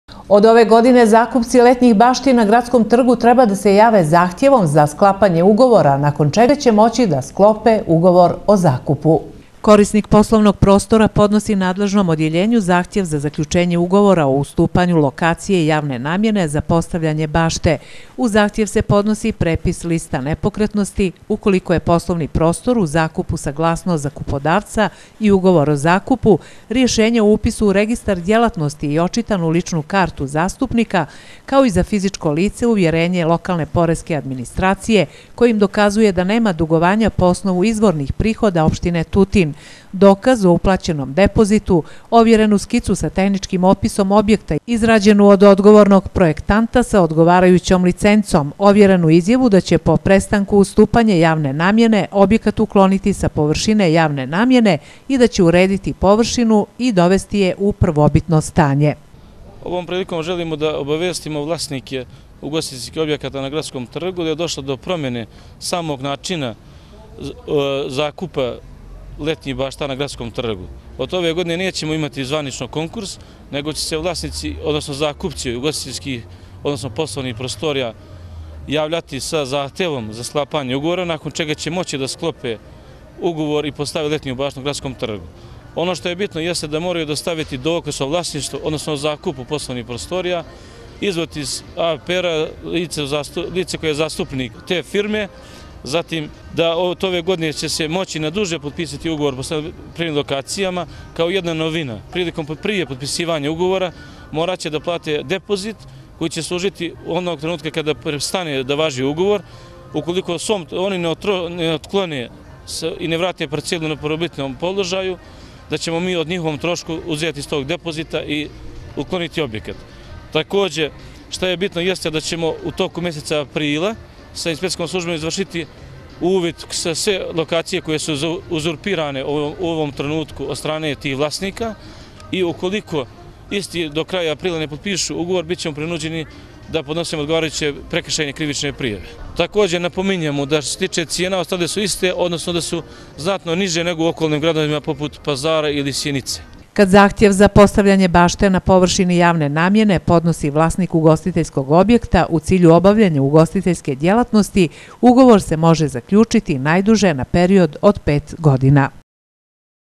Ove godine novine za zakupljivanje letnjih bašti na gradskom trgu
Od ove godine zakupci letnjih bašti na gradskom trgu treba da se jave zahtjevom za sklapanje ugovora, nakon čega će moći da sklope ugovor o zakupu.